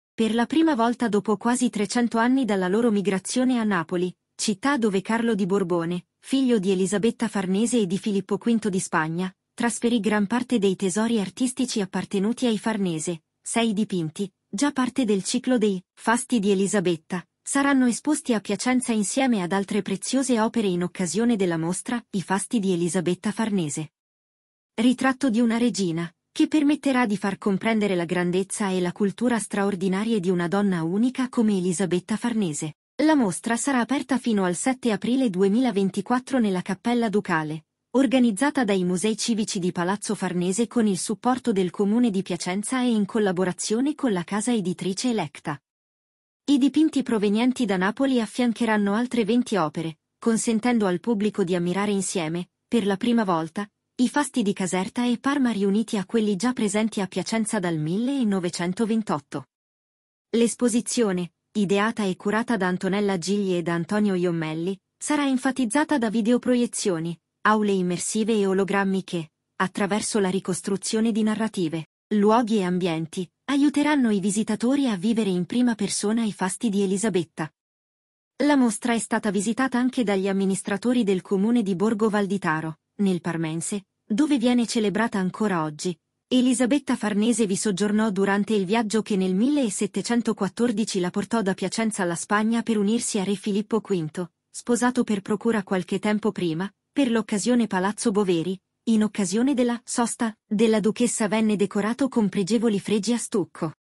I fasti di Elisabetta Farnese
Per la prima volta dopo quasi trecento anni dalla loro migrazione a Napoli - città dove Carlo di Borbone, figlio di Elisabetta Farnese e di Filippoquinto di Spagna, trasferì gran parte dei tesori artistici appartenuti ai Farnese - sei dipinti, già parte del ciclo dei “Fasti di Elisabetta”, saranno esposti a Piacenza insieme ad altre preziose opere in occasione della mostra “I Fasti di Elisabetta Farnese. Ritratto di una Regina”, che permetterà di far comprendere la grandezza e la cultura straordinarie di una donna unica come Elisabetta Farnese.

La mostra sarà aperta fino al 7 aprile 2024 nella Cappella Ducale, organizzata dai Musei Civici di Palazzo Farnese con il supporto del Comune di Piacenza e in collaborazione con la casa editrice Electa.
I dipinti provenienti da Napoli affiancheranno altre venti opere, consentendo al pubblico di ammirare insieme, per la prima volta, i Fasti di Caserta e Parma riuniti a quelli già presenti a Piacenza dal 1928.
L’esposizione, ideata e curata da Antonella Gigli e da Antonio Iommelli, sarà enfatizzata da videoproiezioni, aule immersive e ologrammi che, attraverso la ricostruzione di narrative, luoghi e ambienti, aiuteranno i visitatori a vivere in prima persona i Fasti di Elisabetta.
La mostra è stata visitata anche dagli amministratori del comune di Borgo Val di Taro, nel parmense, dove  viene celebrata ancora oggi, Elisabetta Farnese vi soggiornò durante il viaggio che nel 1714 la portò da Piacenza alla Spagna per unirsi a re Filippo V, sposato per procura qualche tempo prima, per l’occasione Palazzo Boveri,  in occasione della “sosta” della duchessa venne decorato con pregevoli fregi a stucco